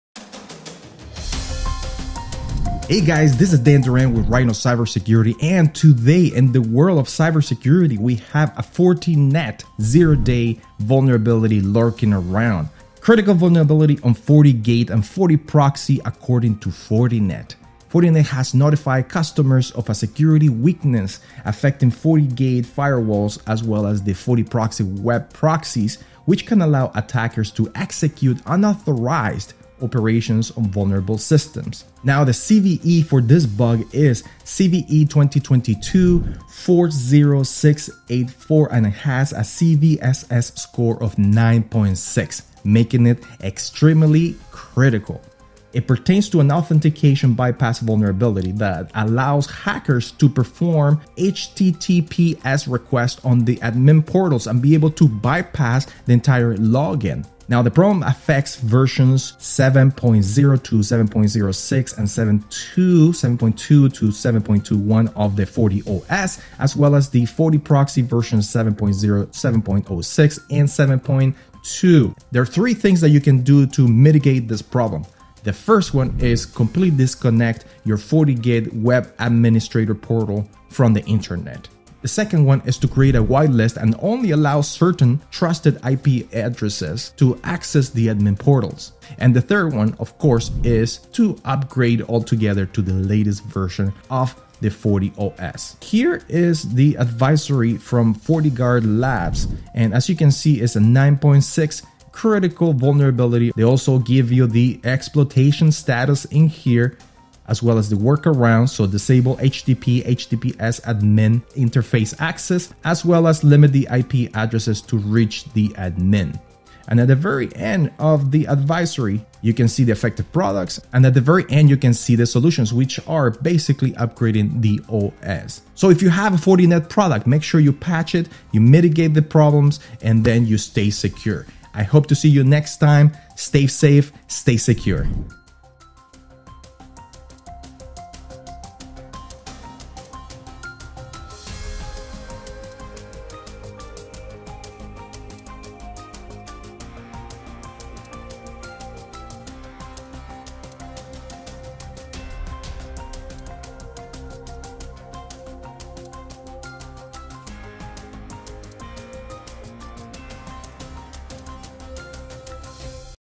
ALERT: Critical Vulnerability on FortiGate and FortiProxy
Vulnerability on FortiGate. Fortinet has notified its customers of a security weakness affecting FortiGate firewalls and FortiProxy web proxies, which can allow attackers to execute unauthorized operations on vulnerable systems.

Vulnerability on FortiGate | The significant bug, identified as CVE-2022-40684 (CVSS score: 9.6), pertains to an authentication bypass vulnerability that allows unauthenticated attackers to perform arbitrary activities on the administrative interface through a carefully crafted HTTP(S) request.

The problem affects the following versions, which have been fixed in FortiOS 7.0.7 and 7.2.2, as well as FortiProxy 7.0.7 and 7.2.1, which were published this week:

FortiOS – Versions 7.0.0 to 7.0.6 and 7.2.0 to 7.2.1
FortiProxy – Versions 7.0.0, 7.0.6, and 7.2.0

Due to the potential for remote exploitation of this problem, Fortinet strongly advises all customers with vulnerable versions to execute an immediate upgrade, the firm said in a tweet published by security researcher Gitworm.

Fortinet is aware of an incident in which this vulnerability was exploited and recommended quickly checking your systems against the following indicator of compromise in the device’s logs: user=’Local Process Access,’ the firm said in a warning.